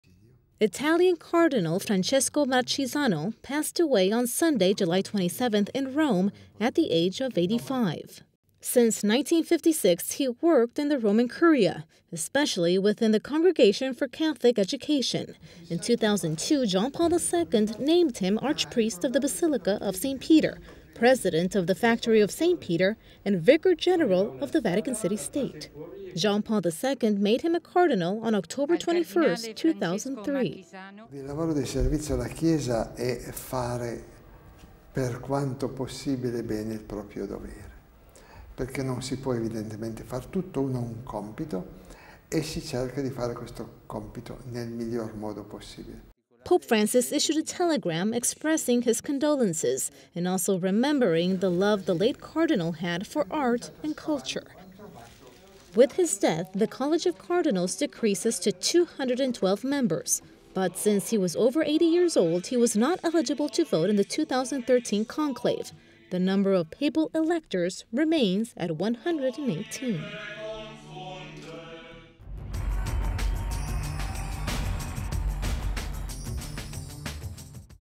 Italian Cardinal Francesco Marchisano passes away | Vatican
Italian Cardinal Francesco Marchisano passed away on Sunday, July 27, in Rome, at the age of 85.

YJA/RCA
RR
FA .